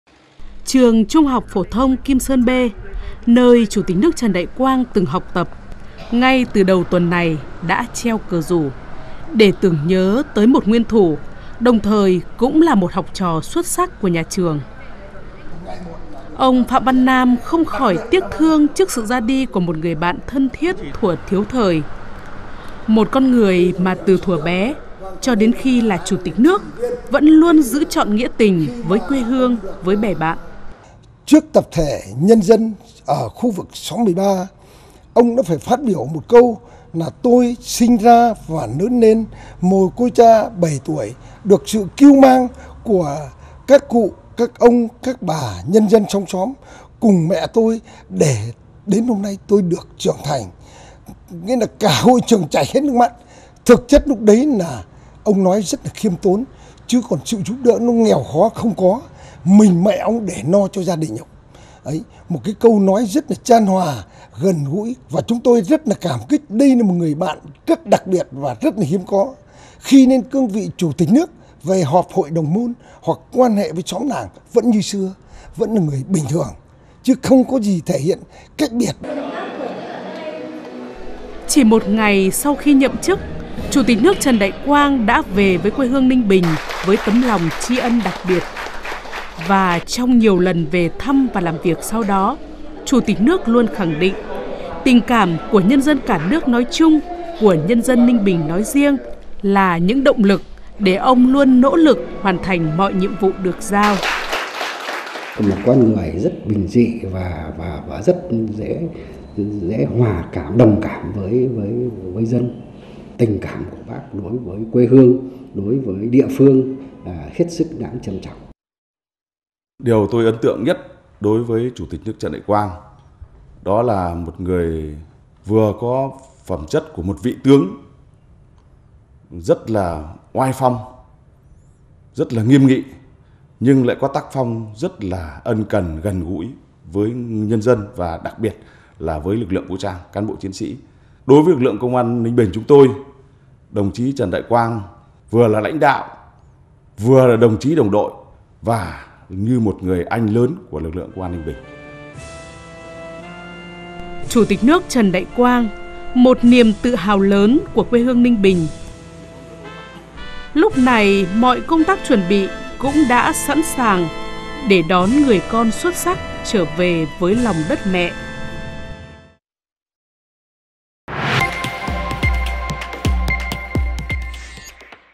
Chủ tịch nước Trần Đại Quang - Người trọn nghĩa, vẹn tình với quê hương | VTV24
Chủ tịch nước Trần Đại Quang là người con ưu tú, xuất sắc của Đảng bộ, chính quyền, nhân dân Tỉnh Ninh Bình. Sự ra đi của Chủ tịch nước là mất mát lớn đối với quê hương.